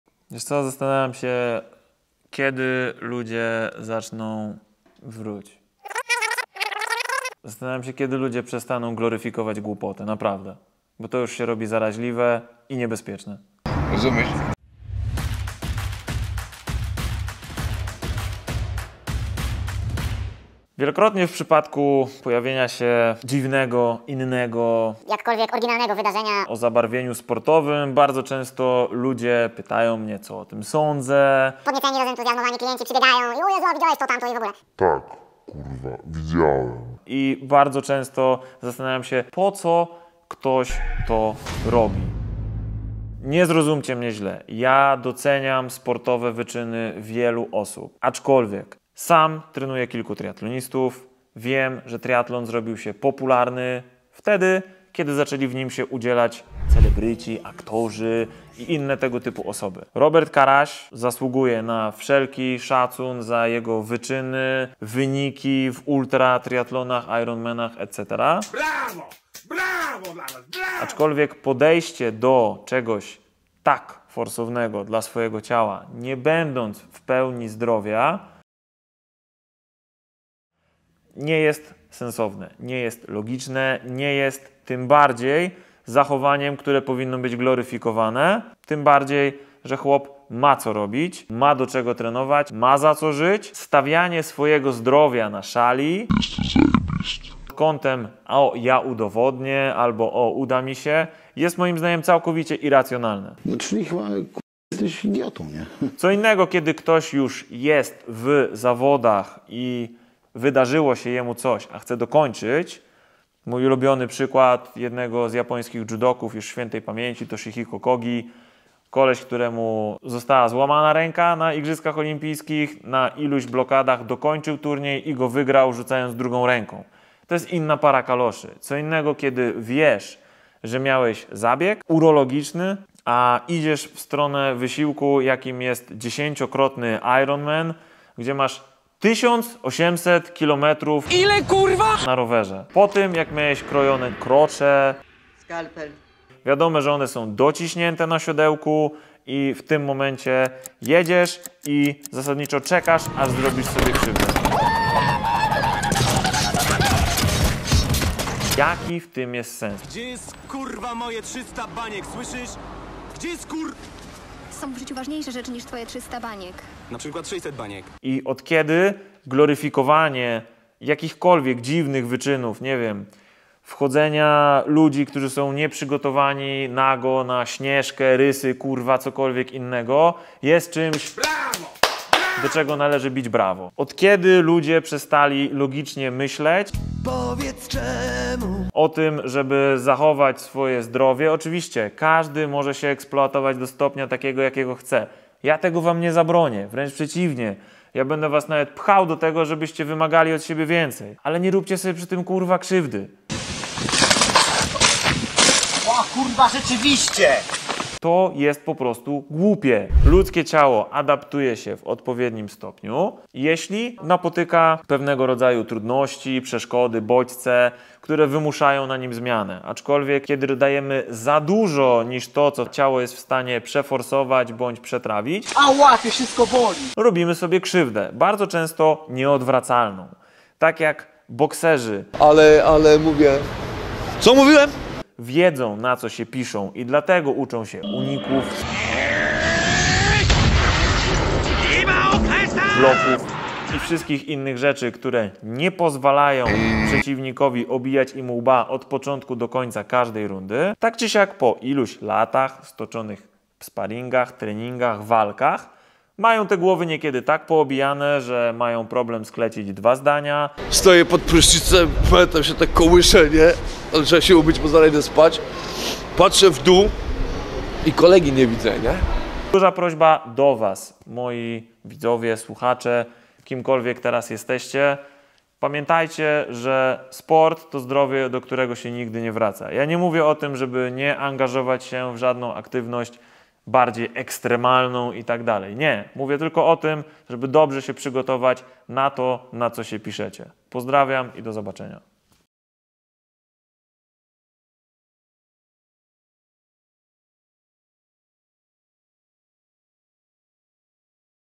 SPORT TO ZDROWIE / CZY NA PEWNO? / MOJA REAKCJA NA OSTATNIE WYDARZENIA
Witajcie

Temat dość mocny bo żyła nim cała Polska.

Szanuję wszystkich sportowców ale z drugiej strony wzorce jakie powinny być pokazywane i przekazywane powinny zachęcać do szanowania i dbania o swoje ciało.

Ryzykowanie tak dużych obciążeń będąc nie w pełni zdrowym jest mało sensowne i rentowne.
Zarówno z punktu widzenia kariery sportowej jak i reszty życia.

Zbigniew w formie i zaproponował nową formę montażu reportaży i monologów.
Dajcie znać co o tym sądzicie.

Pozdrawiam
Maciej

PS. Jak uważacie, że temat ważny - proszę udostępnijcie dalej